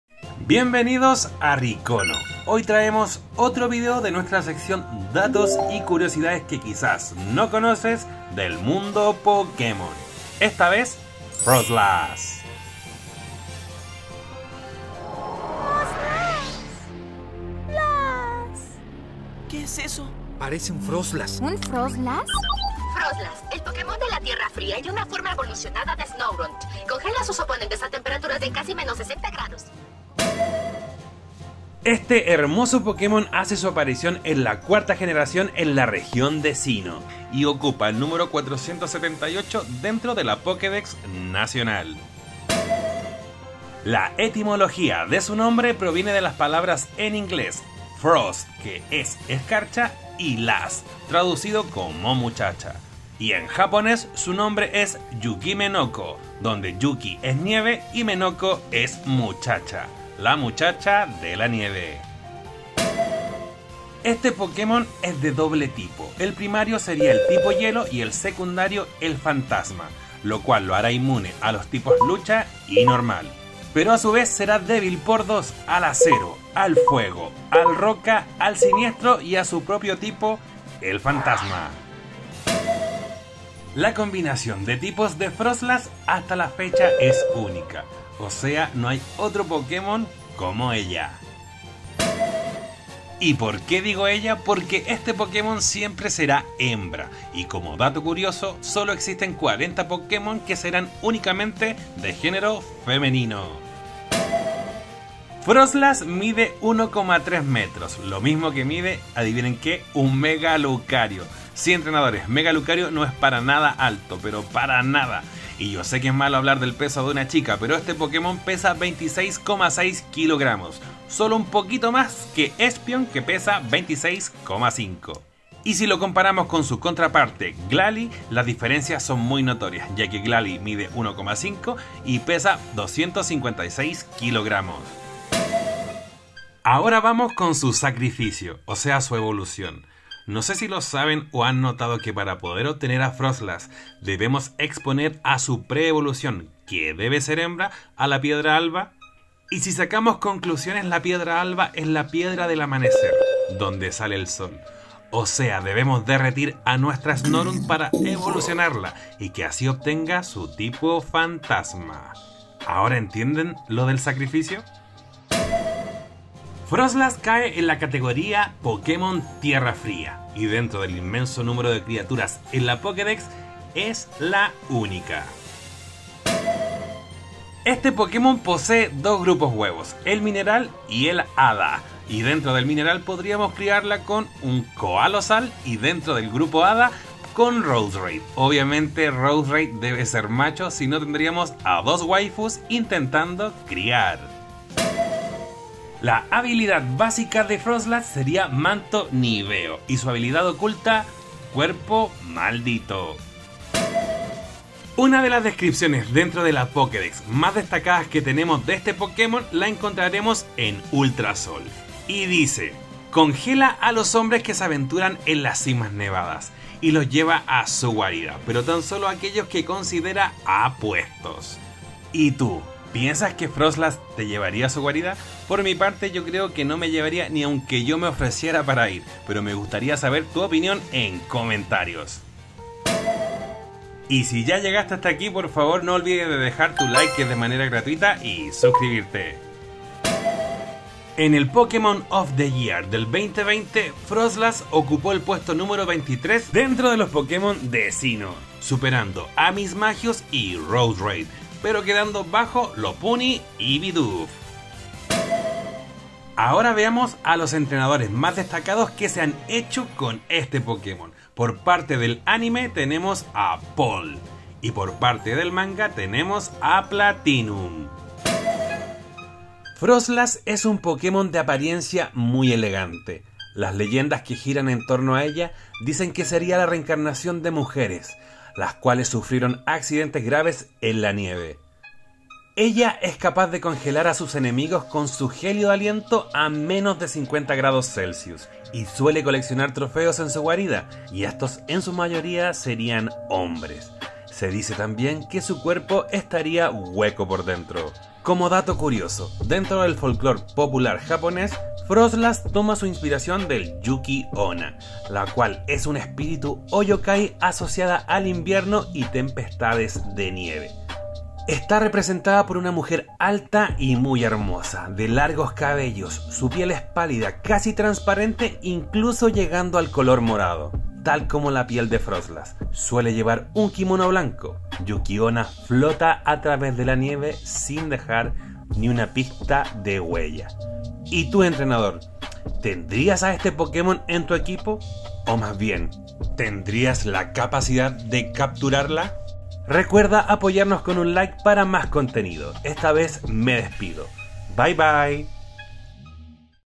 ✨FROSLASS✨- Datos y Curiosidades que Todo entrenador debe Saber - POKÉMON
TENEMOS UNA NUEVA SECCION EN EL CANAL "DATOS Y CURIOSIDADES QUE QUIZAS NO SABÍAS DE TUS POKEMON FAVORITOS"
CONTINUAMOS CON FROSLASS, EL POKÉMON TIPO HIELO / FANTASMA AMADA POR MUCHOS

Recuerda dejar tu Like y seguirnos en nuestras RRSS.